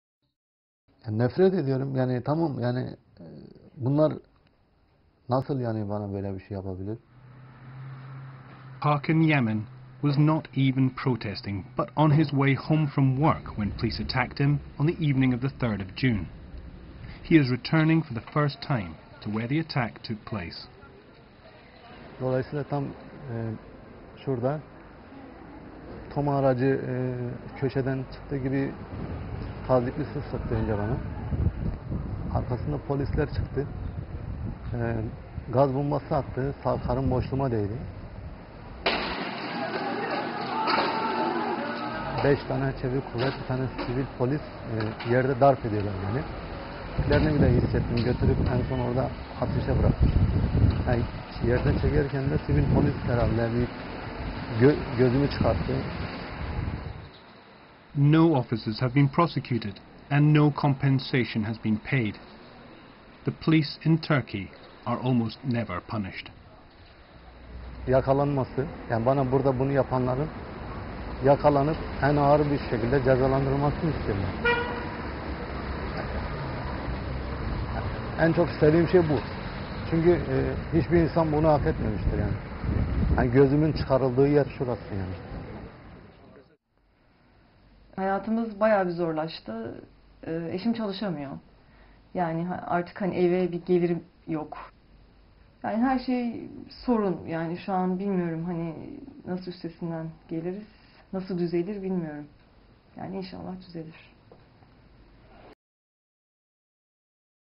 Abuso policial na Turquia: A História de Hakan Yaman
Hakan Yaman passava junto às manifestações deste verão na Turquia quando foi espancado pela polícia. Testemunho na primeira pessoa.